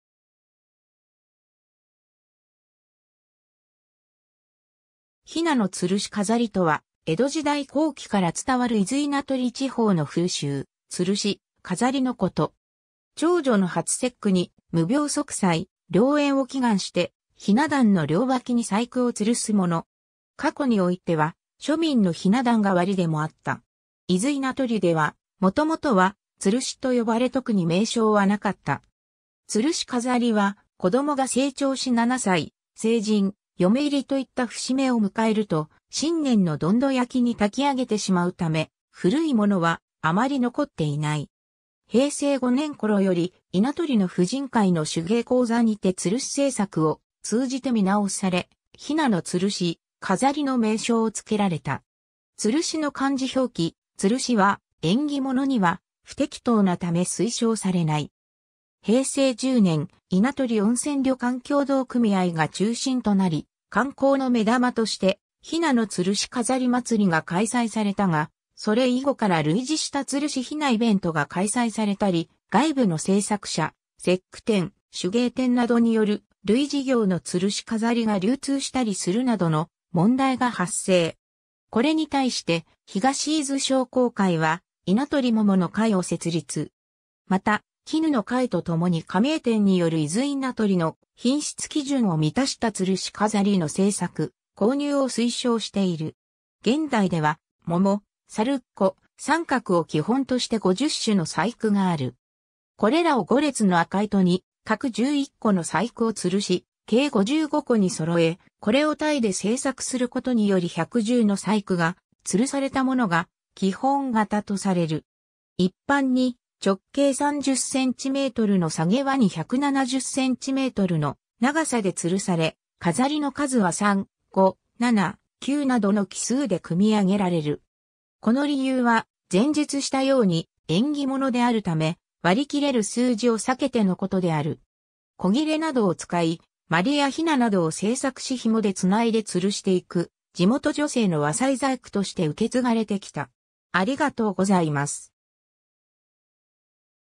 雛のつるし飾り
雛のつるし飾り（ひなのつるしかざり）とは、江戸時代後期から伝わる伊豆稲取地方の風習、吊るし飾りのこと。
長女の初節句に、無病息災、良縁を祈願して、雛壇の両脇に細工を吊すもの。
過去においては庶民の雛壇代りでもあった。
 伊豆稲取では、もともとはツルシと呼ばれ特に名称はなかった。
つるし飾りは、子供が成長し7歳、成人、嫁入りといった節目を迎えると、新年のどんど焼きに焚きあげてしまうため、古いものはあまり残っていない。
平成5年頃より稲取の婦人会の手芸講座にてツルシ製作を通じて見直され、「雛のつるし飾り」の名称をつけられた。
つるしの漢字表記「吊るし」は、縁起物には不適当なため推奨されない。
 平成10年、稲取温泉旅館協同組合が中心となり観光の目玉として雛のつるし飾りまつりが開催されたが、それ以後から類似したつるし雛イベントが開催されたり、外部の製作者、節句店、手芸店などによる類似異形のつるし飾りが流通したりするなどの問題が発生。
これに対して、東伊豆商工会は“稲取ももの会”を設立。
また“絹の会”とともに加盟店による伊豆稲取の品質基準を満たしたつるし飾りの製作、購入を推奨している。
 現代では、桃（長寿）、猿っ子（魔除け）、三角（薬袋香袋）を基本として50種の細工がある。
これらを5列の赤糸に各11個の細工をつるし計55個にそろえ、これを対で製作することにより110の細工がつるされたものが基本型とされる。
 一般に直径30cmのさげわに170cmの長さで吊るされ、飾りの数は3、5、7、9などの奇数で組み上げられる。
この理由は、前述した様に縁起物であるため、割り切れる数字（偶数）を避けてのことである。
古裂などを使い、鞠や雛などを制作しひもで繋いでつるしていく、地元女性の和裁細工として受継がれて来た。